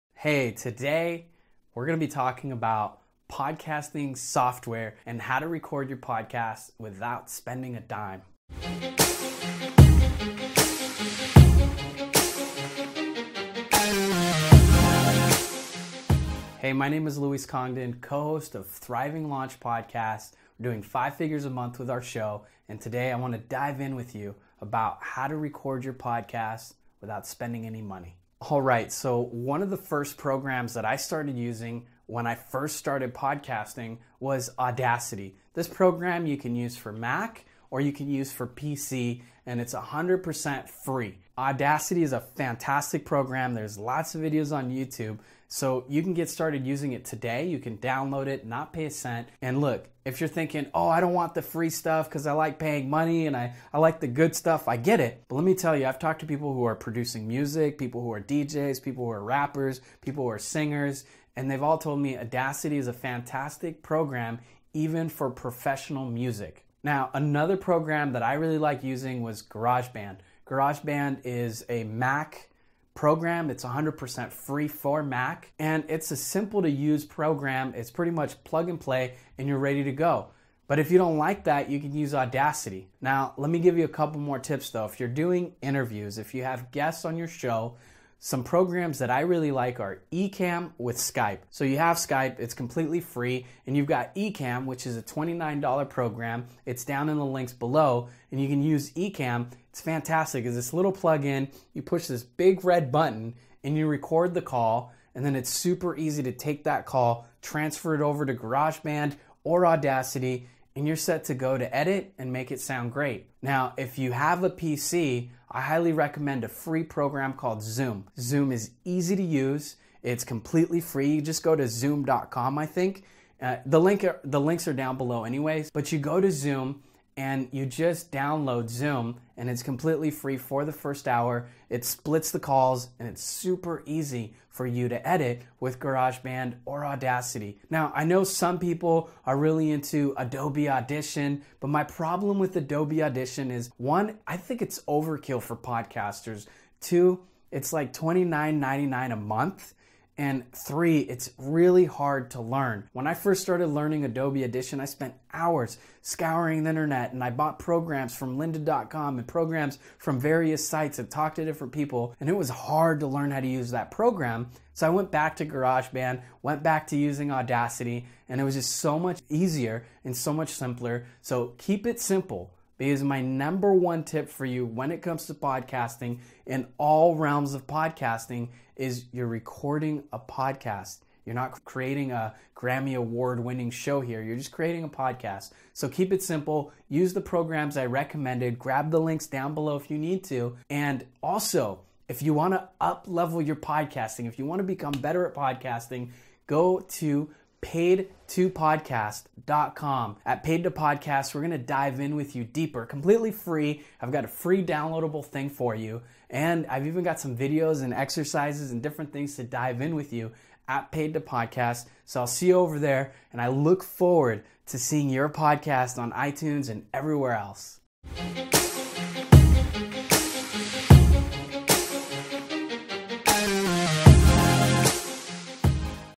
Podcast Recording Software
In this video, I'm going to talk about Podcast Recording Software. Here is the FREE guide that goes with this video

The first thing you need to learn about Podcast Recording Software is that there are so many programs you can choose from. And I’m going to share some of what I think are the best without letting you spend any money.

Let me enumerate the following: Audacity, Garage Band, Ecamm and Zoom. These are my recommended programs and I will discuss their pros and cons. Throughout the video, I’ll also be telling my personal experience with each program and how these Podcast Recording Software helped me improve.

Once you've established the Podcast Recording Software that you want to use, podcasting will be easier to do. You’ll be able to record without any hassle and the finish product is also executed well.

So keep it simple because my number one tip for you when it comes to podcasting and all realms of podcasting is you’re recording a podcast. You're not creating a Grammy award winning show here. You're just creating a podcast.

Follow Everywhere And Stay In Touch!